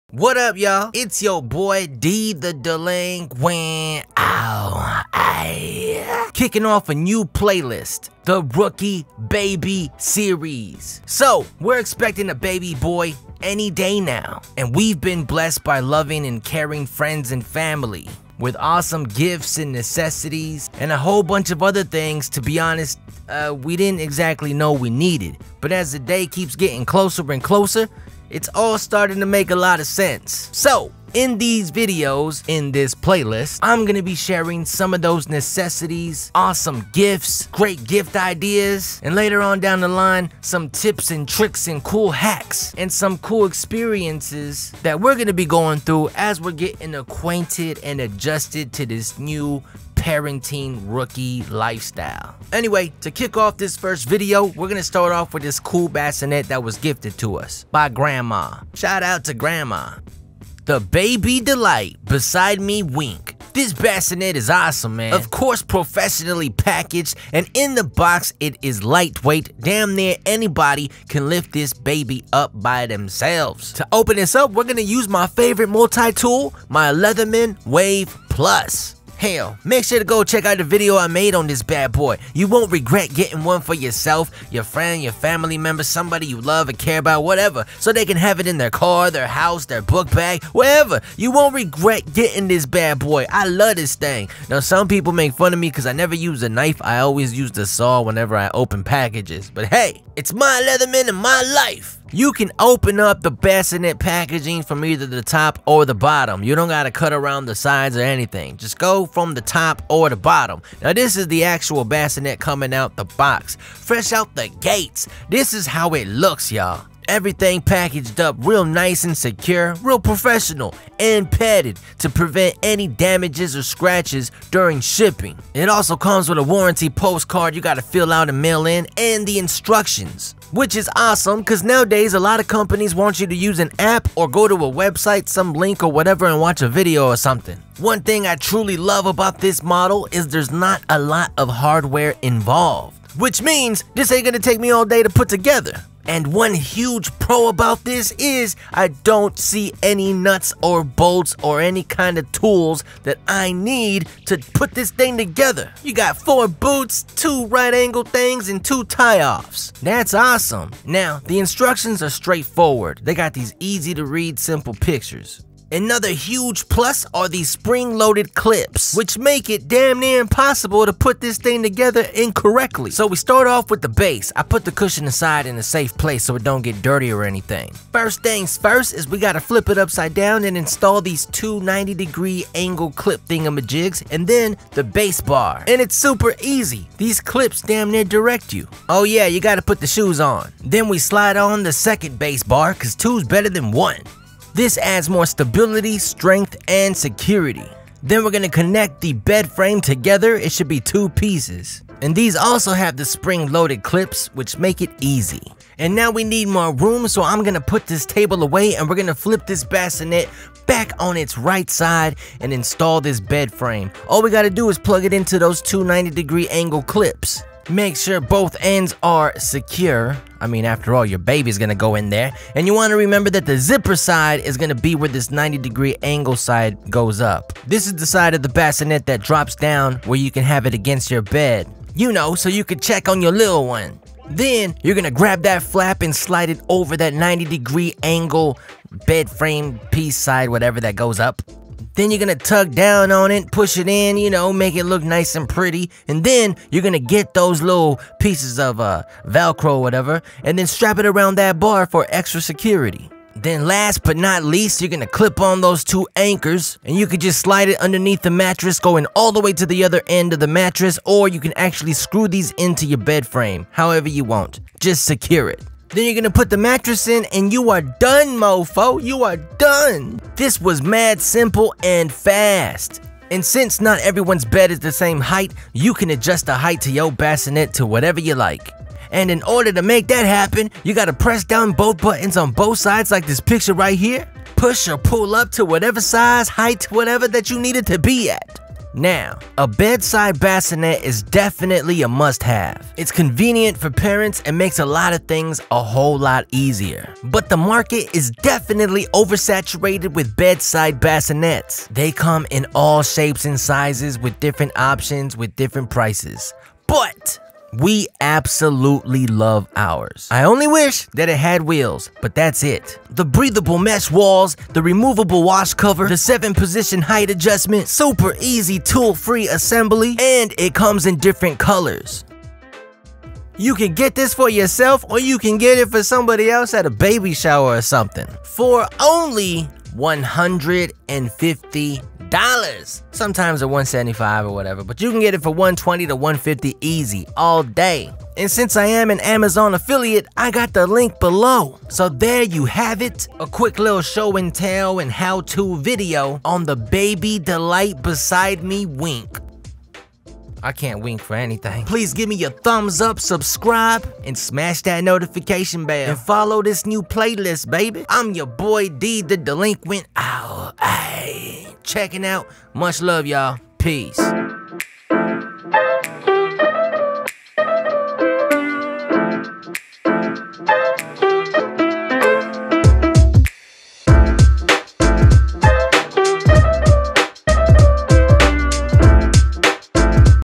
Baby Delight Beside Me Wink Bassinet Review
The Rookie Baby Series
This review kicks off the new playlist for the channel. I'll share/review all items BABY and will also share some extra content related to Baby thangz.
This bassinet by Baby Delight is a good one to purchase for your own baby or for you to purchase for someone else's baby.
Baby Delight Beside Me Wink

Here's a few more Bassinet Options that are similiar
Similar Bassinet w/Wheels
An Even Closer than bedside bassinet

LeatherMan Wave+

Sound and Equipment Used for this video
Neuman TLM 103
Scarlet Audio Int 3rd Gen
GoPro 8
GoPro Max 360